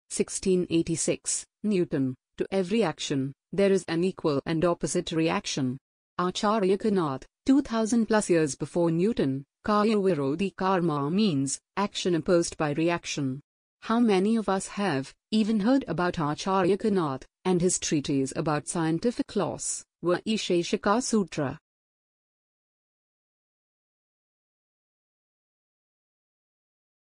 The Newton's Law exists 2000+ years before newton |Who Discovered Newton's Law before Newton#shorts
The Newton's Law exists 2000+ years before newton. | Who Discovered Newton's Law before Newton.

Source - Vaisesika Sūtra